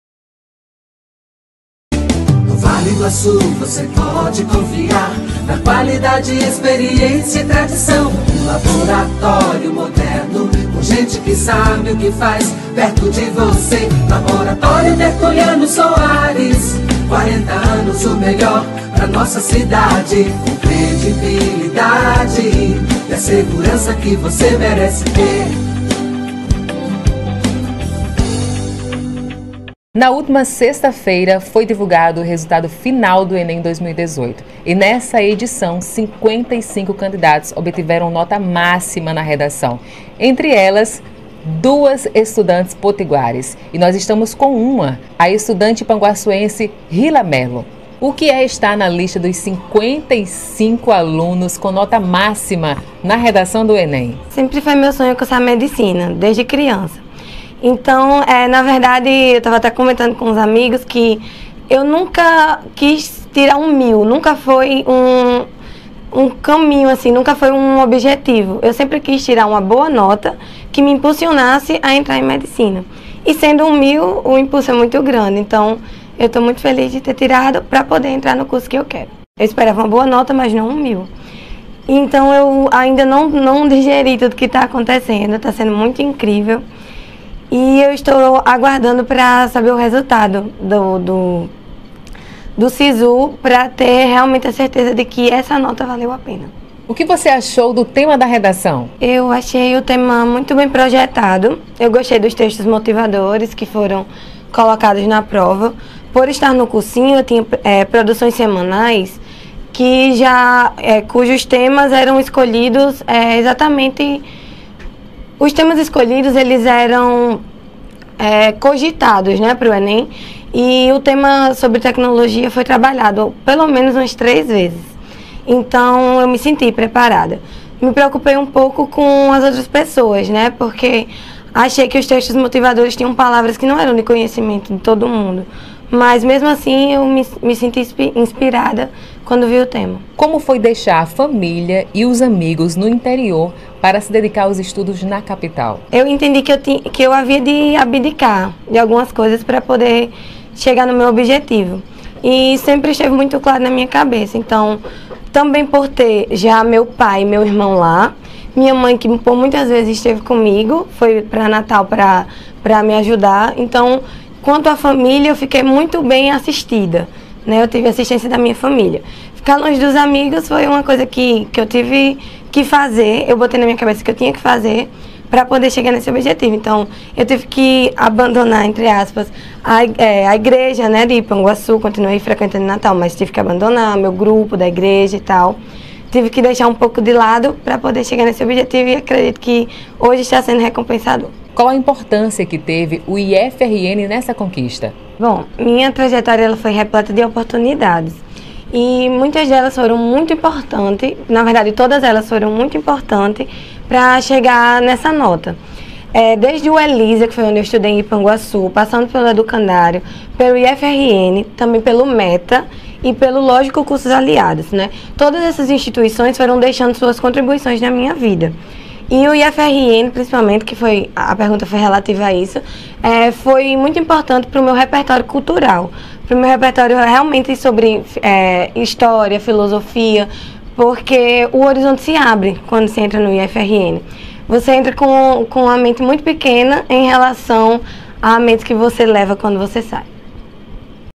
“Eu esperava uma boa nota, mas não mil”, diz Ipanguaçuense nota 1000 no ENEM
A ipanguaçuense Rylla Melo está na relação dos 55 estudantes do Brasil que obtiveram nota mil na redação do ENEM 2018. O resultado do Exame Nacional do Ensino Médio foi divulgado na última sexta-feira, dia 18 de janeiro. Rylla falou sobre o sonho que tem de cursar medicina que agora está mais perto e demonstrou surpresa ao saber que alcançou a pontuação máxima na redação.
Ela afirma que “esperava uma boa nota”, mas não tinha ideia que essa nota fosse a maior.
No Rio Grande do Norte além de Rylla Melo, a estudante mossoroense Carolina Mendes Pereira também alcançou nota máxima na redação.